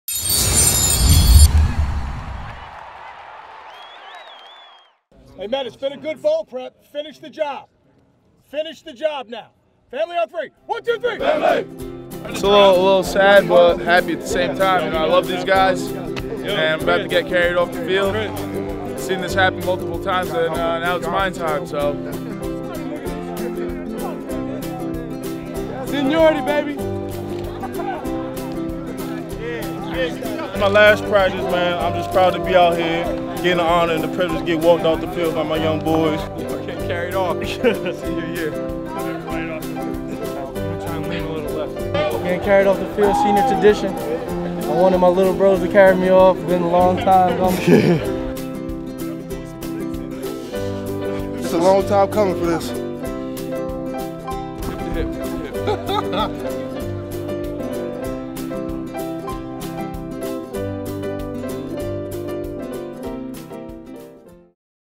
.@RVisionRU Exclusive: @RFootball Seniors Get Carried Off Field
In true RFootball tradition, the senior class was carried off the field by the underclassmen following the final practice of the season at Ford Field.